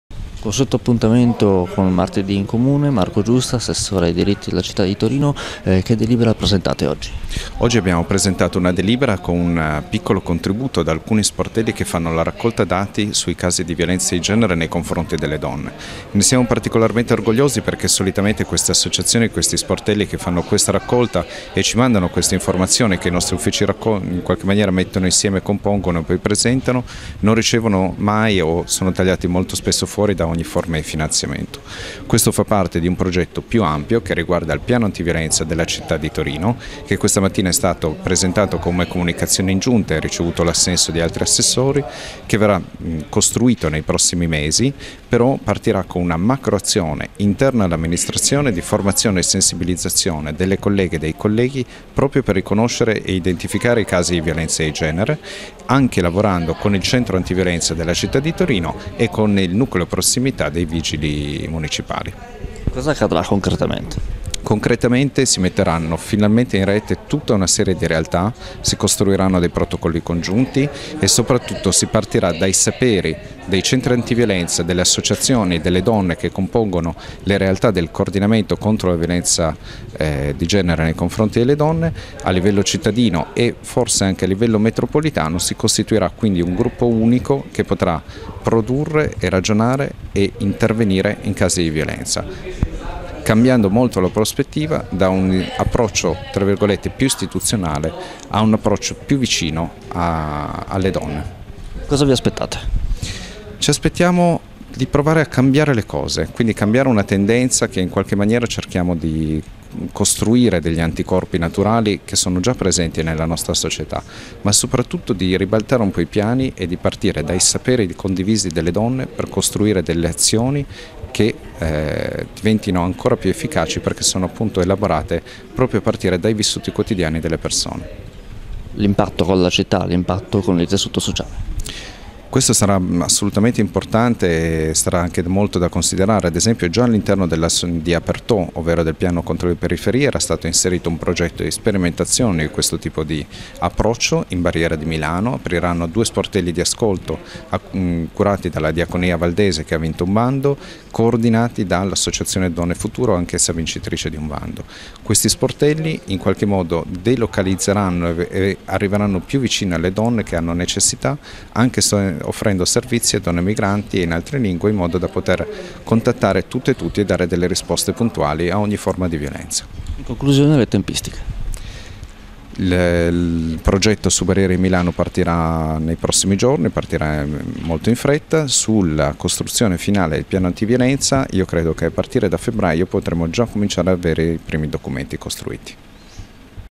TORINO: Presentato un piano anti violenza - 12.12.2017 GRP tv
Nuovo appuntamento con "Martedí in Comune". Marco Giusta, assessore ai Diritti della Città di Torino, presenta un piano anti violenza.